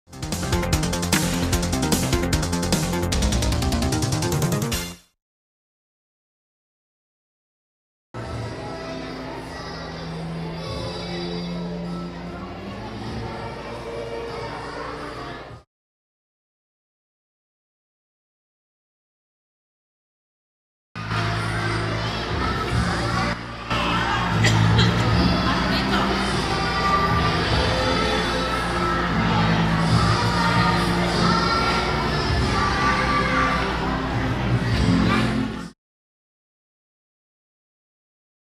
Celebran escuelas el Día de las Naciones Unidas
Sin el ya tradicional desfile por las calles, alumnos de las escuelas celebran el dia de las Naciones Unidas dentro las instituciones.